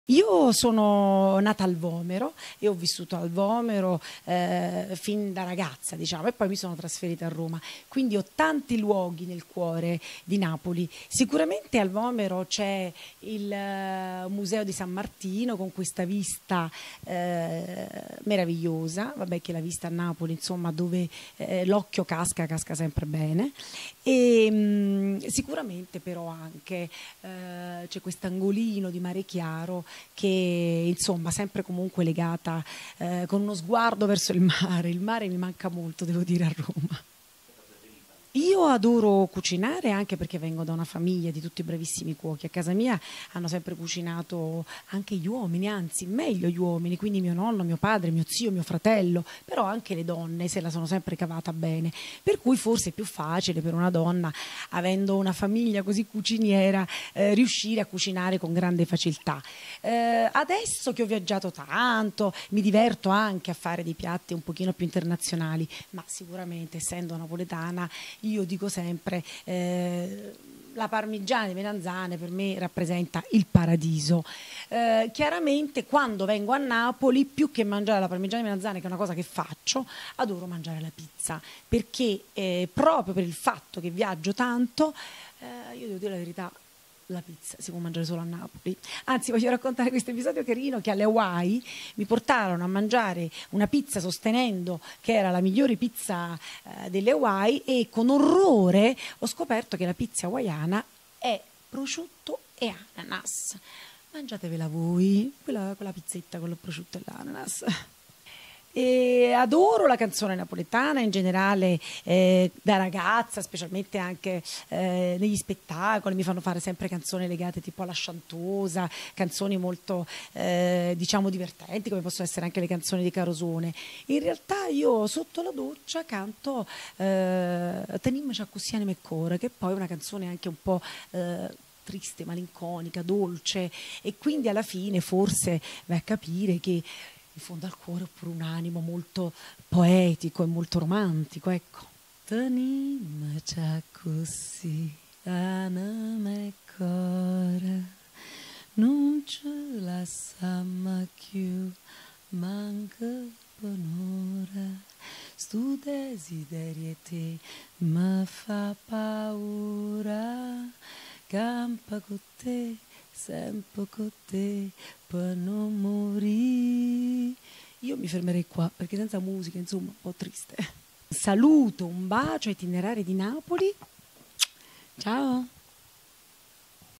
Tosca D'Aquino
Abbiamo incontrato Tosca D'Aquino, attrice ma sopratutto napoletana verace. Tosca D'Aquino ha raggiunto la notorietà con i film di Leonardo Pieraccioni nel 1996 dopo esser stata diretta da grandi registi come Nanni Loy ed Ettore Scola. Simpatica e disponibile (ma d'altronde è napoletana) ci ha intrattenuto cantando per noi.